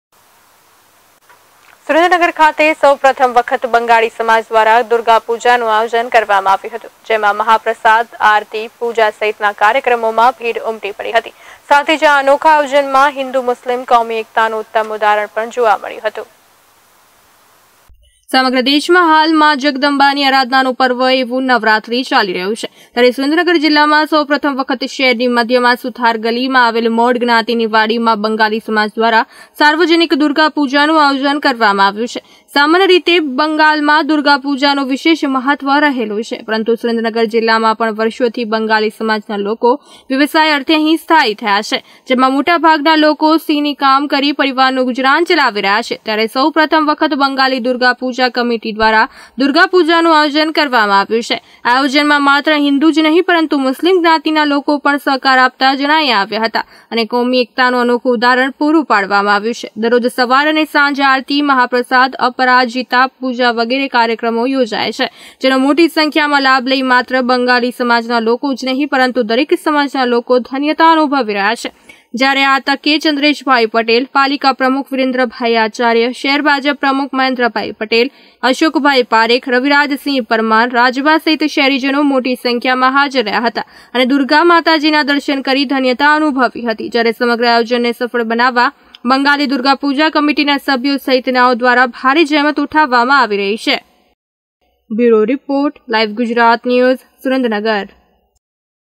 સુરેન્દ્રનગર: બંગાળી સમાજ દ્વારા દુર્ગા પૂજા
સુરેન્દ્રનગર ખાતે સૌ પ્રથમ વખત બંગાળી સમાજ દ્વારા દુર્ગા પૂજાનું આયોજન કરવામાં આવ્યું હતું. જેમાં મહા પ્રસાદ, આરતી, પૂજા સહિતના કાર્યક્રમોમાં ભીડ ઉમટી પડી હતી. સાથે જ આ અનોખા આયોજનમાં હિન્દુ-મુસ્લિમ કોમી એકતાનું ઉત્તમ ઉદાહરણ પણ જોવા મળ્યું હતું.